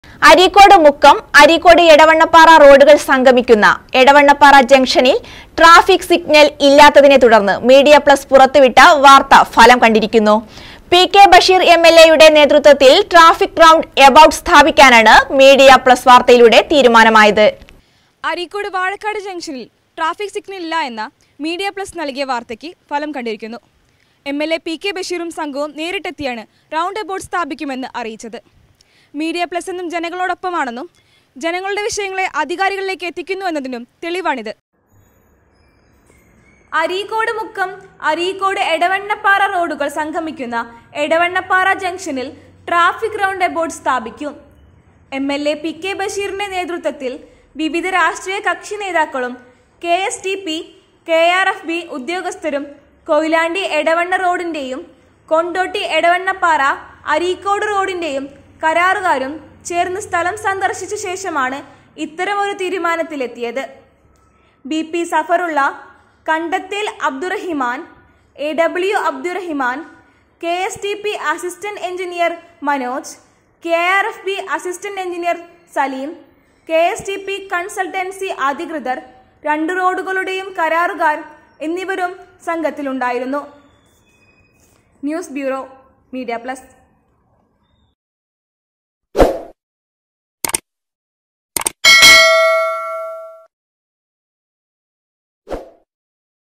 മീഡിയ പ്ലസ് റിപ്പോര്ട്ടിനെ തുടര്ന്ന് ട്രാഫിക് സിഗ്നല് സ്ഥാപിക്കാന് ഒരുങ്ങി അധികാരികള്.
തിരക്കേറിയ അരീക്കോട് എടവണ്ണപാറ,അരീക്കോട്-മുക്കം  ജംഗ്ഷനില് ട്രാഫിക് സിഗ്നല് ഇല്ലെന്ന മീഡിയ പ്ലസ് റിപ്പോര്ട്ടിനെ തുടര്ന്ന് ട്രാഫിക് സിഗ്നല് സ്ഥാപിക്കാന് ഒരുങ്ങി അധികാരികള്.